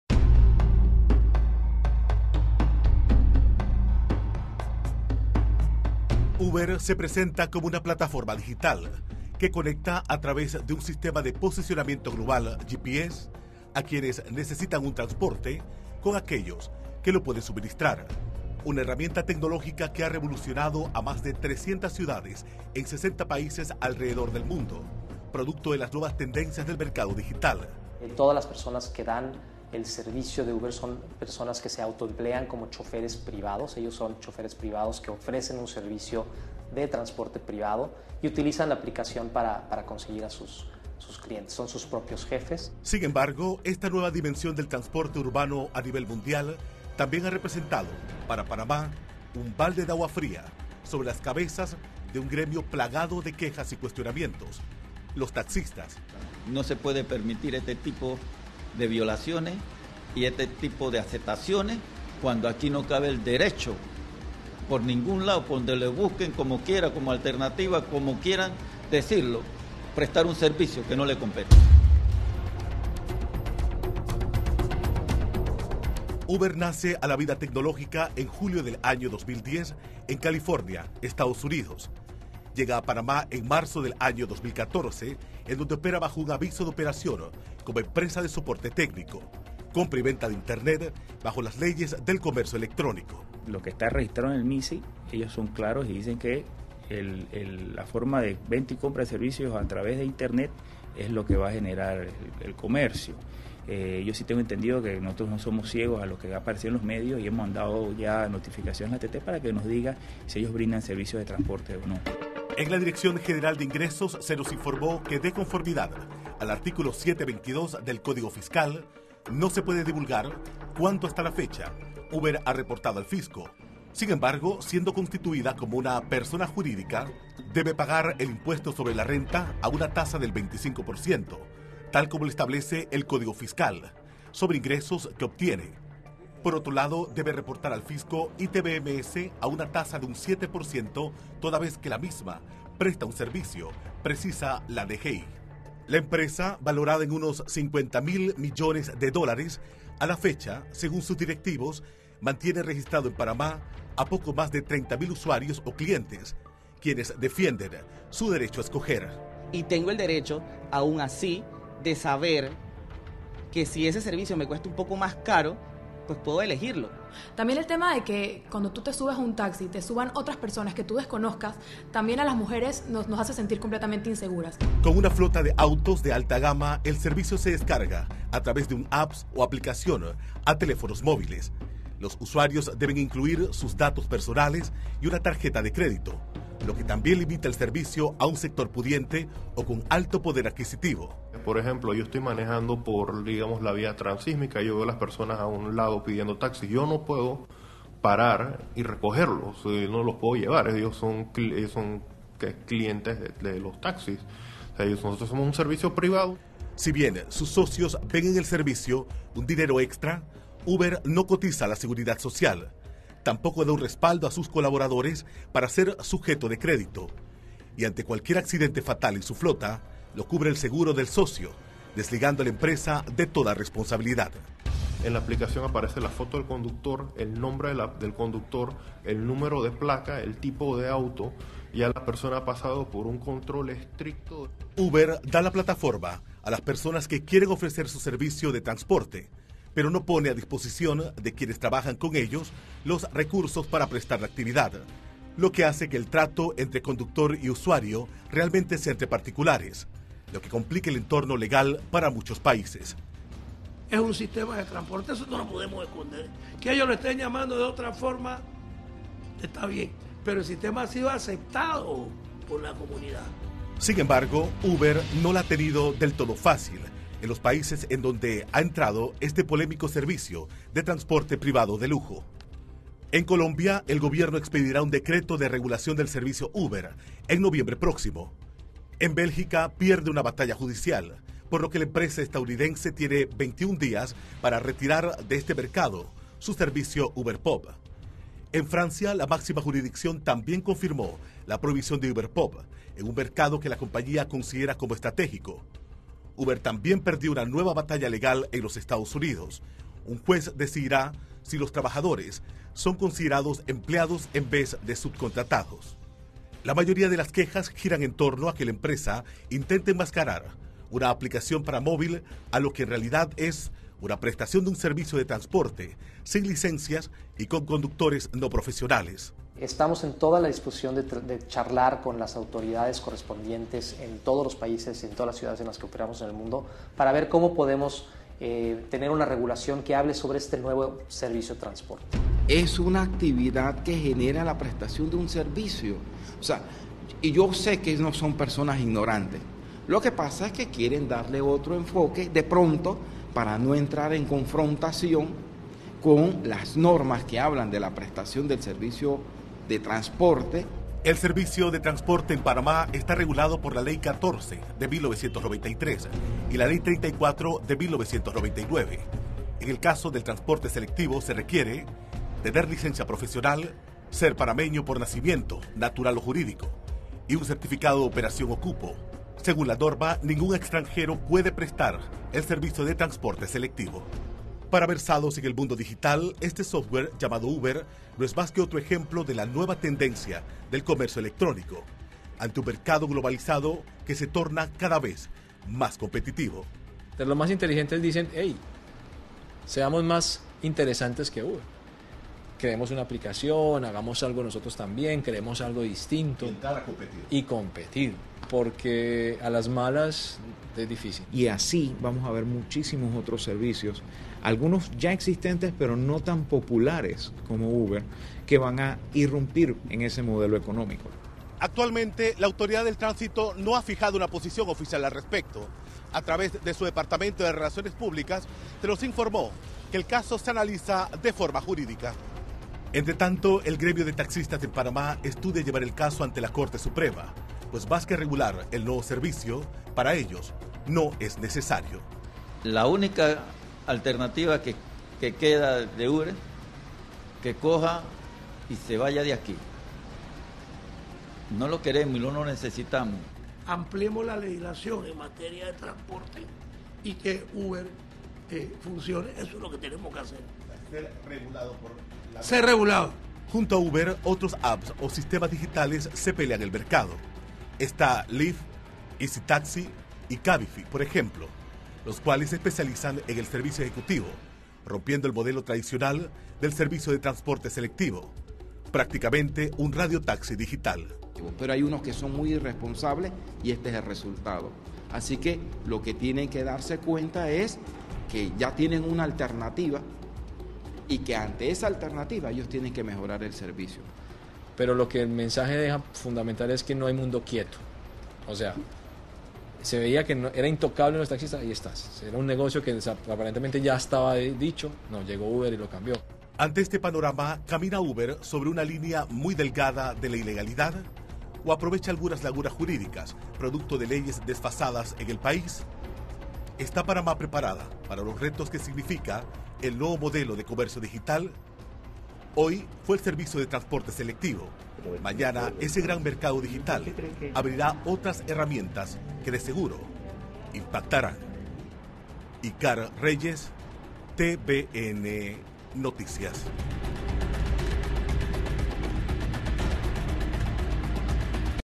El polémico servicio Uber y sus implicaciones en Panamá
Tuve la oportunidad de participar en una entrevista para TVN sobre UBER en Panamá, junto a otros profesionales que usamos Uber tanto en Panamá como en otros países.